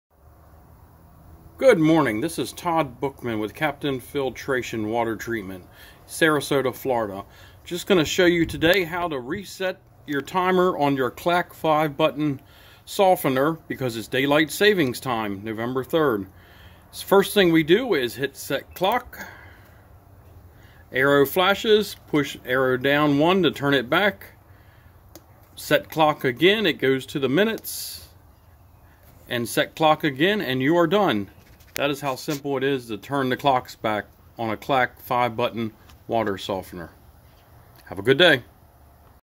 Clack softener timer reset
Turn the clock back for day light savings